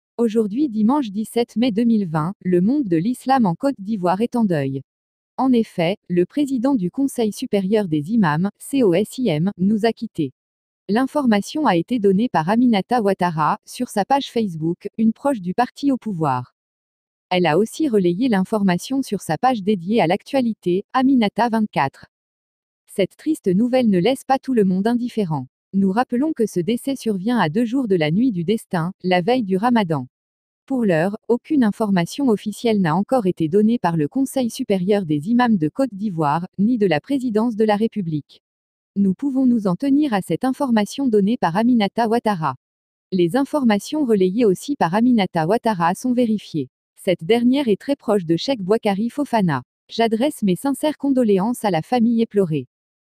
DECES DE CHEICK BOIKARY FOFANA PRESIDENT DE COSIM // IB 2KONE
Aujourd'hui dimanche 17 mai 2020, le monde de l'islam en Côte d'Ivoire est en deuil. En effet, le Président du Conseil Supérieur des Imams
( COSIM ) nous a quitté.

         Cette triste nouvelle ne laisse pas tout le monde indifférent. Nous rappelons que ce décès survient à 2 jours de la nuit du destin, la veille du Ramadan. Pour l'heure, aucune information officielle n'a encore été donné par le Conseil Supérieur des Imams de Côte d'Ivoire, ni de la Présidence de la République.

      Nous pouvons nous en tenir à cette information donnée par Aminata Ouattara. Les informations relayées aussi par Aminata Ouattara sont vérifiés. Cette dernière est très proche de Cheick Boikary Fofana.

J'adresse mes sincères condoléances à la famille éplorée.